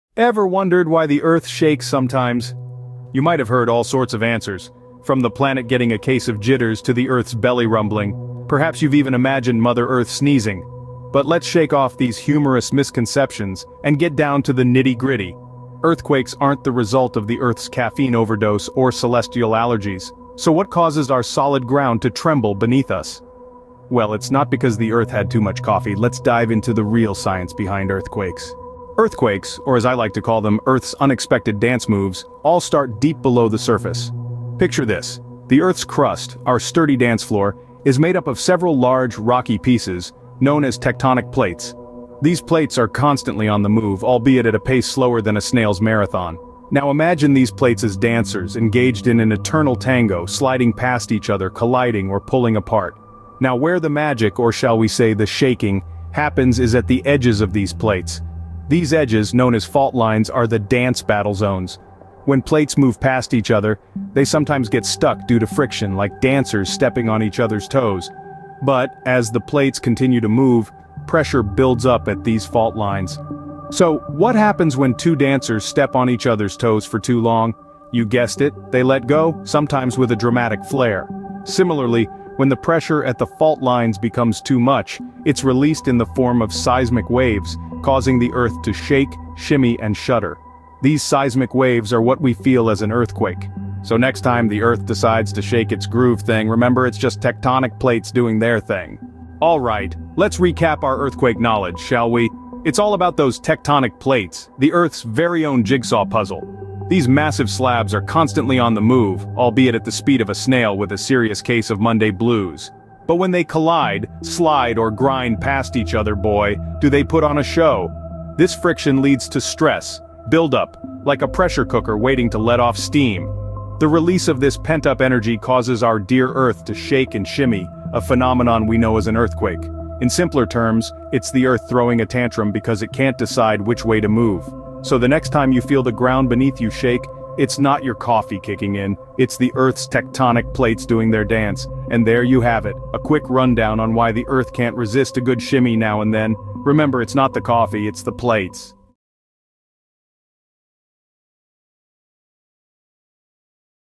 How Do Earthquakes Happen?
🌍 Explore the seismic mysteries of our planet in this illuminating YouTube video. Join us as we delve deep into the Earth's crust to uncover the forces behind earthquakes. From tectonic plate movements to the release of accumulated energy, learn how these natural phenomena shape our world and impact communities worldwide. With stunning visuals and expert insights, this video offers a compelling journey into the heart of Earth's seismic activity. Don't miss out on this eye-opening exploration—hit play now and embark on a seismic adventure! 🌋🔍

This video is made with InVideo AI!